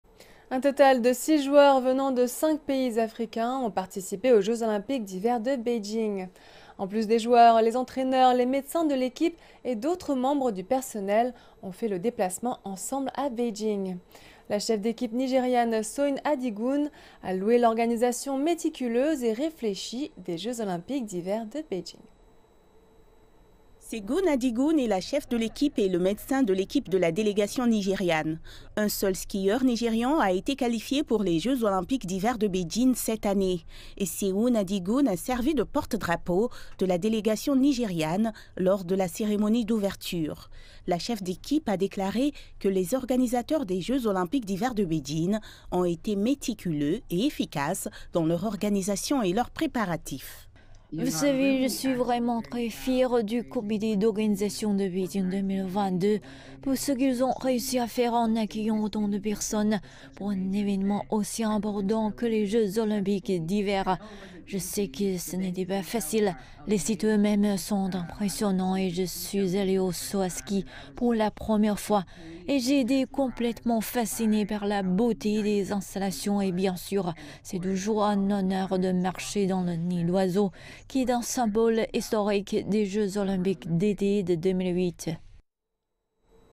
Beijing 2022 : Cheffe d'équipe du Nigéria-le travail des organisateurs est remarquable
Un total de 6 joueurs venant de 5 pays africains ont participé aux Jeux olympiques d'hiver de Beijing. En plus des joueurs, les entraîneurs, les médecins de l'équipe et d'autres membres du personnel ont fait le déplacement ensemble à Beijing. La cheffe d'équipe nigériane Seun Adigun a loué l'organisation méticuleuse et réfléchie des Jeux olympiques d'hiver de Beijing.